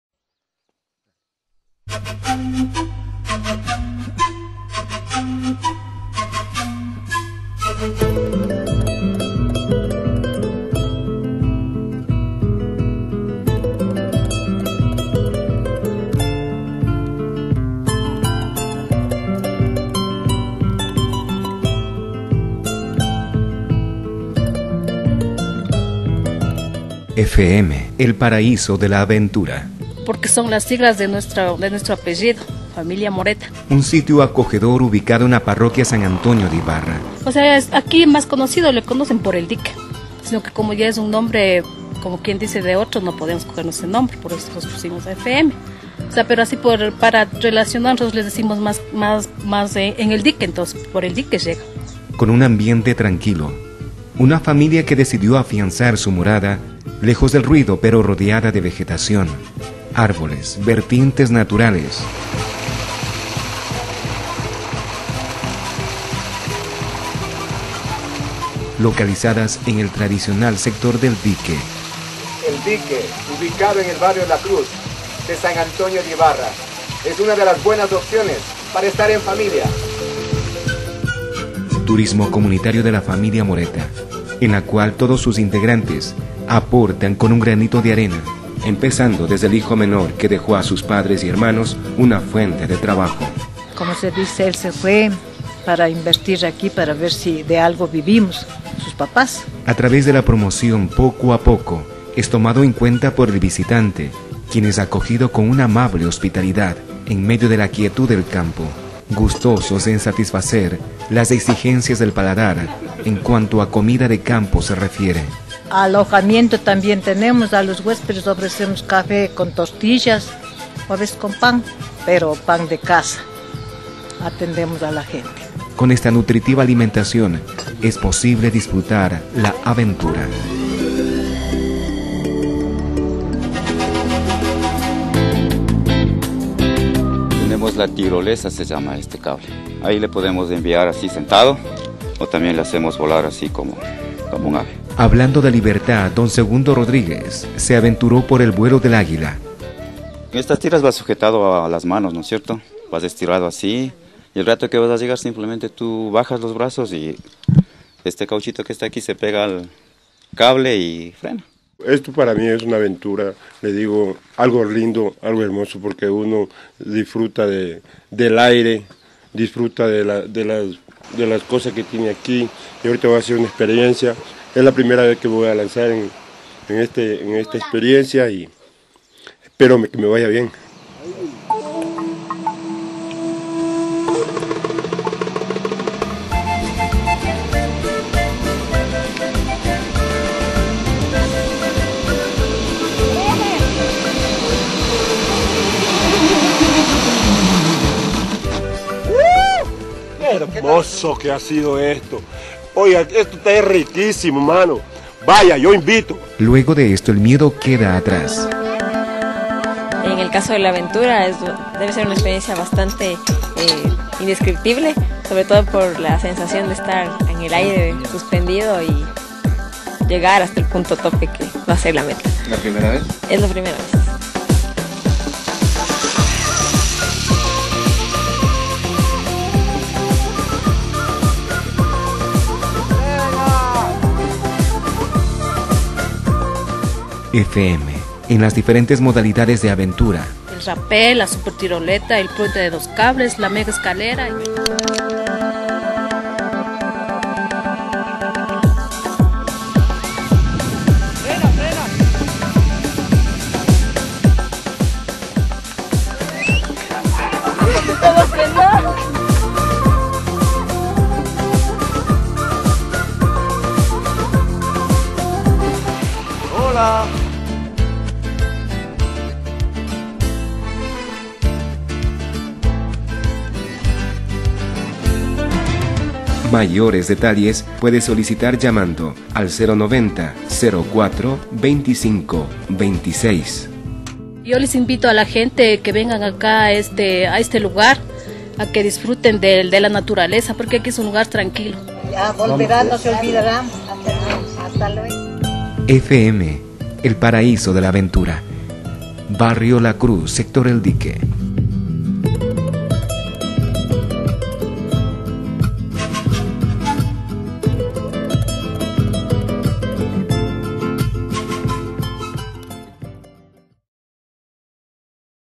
FM PARAÍSO, EMPRENDIMIENTO TURÍSTICO EN SAN ANTONIO DE IBARRA
El emprendimiento turistico "FM Paraíso" perteneciente a  la parroquia de San Antonio de Ibarra, ubicado en el sector del Dique, tiene a disposicion el mejor ambiente natural, juegos de aventura y la atención amable de la familia Morerta, que les brindará lo posibilidad de pasar momento gratos.